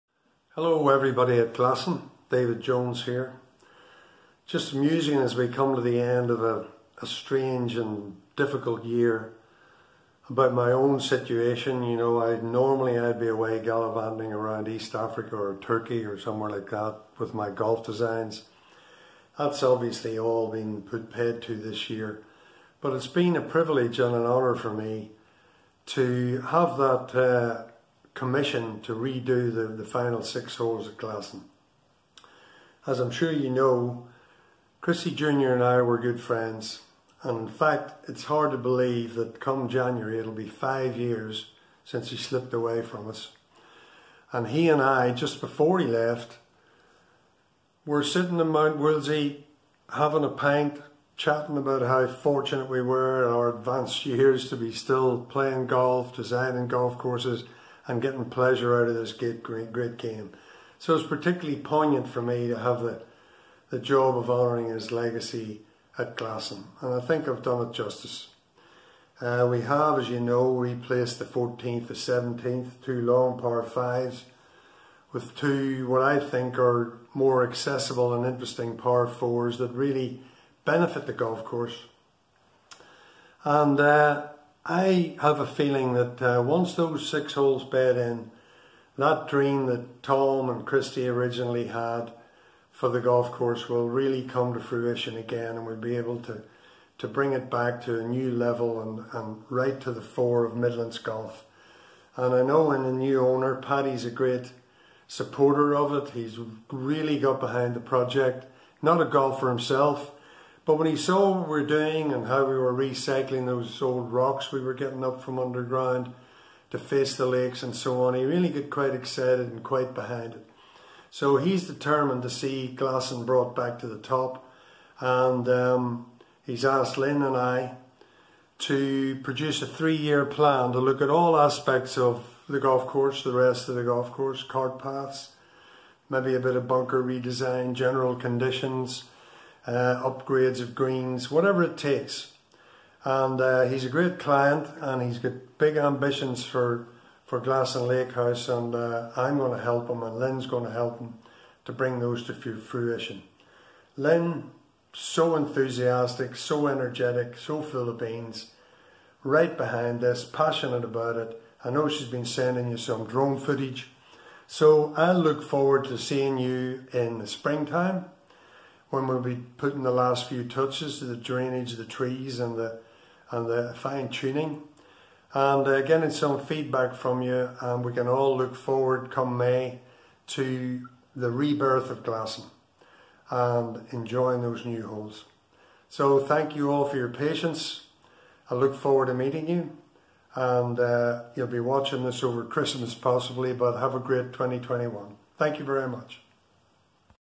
Golf Course Designer, David Jones
Golf Course Designer David Jones sending message of updates and New Year Wishes to Glasson Golf Club members.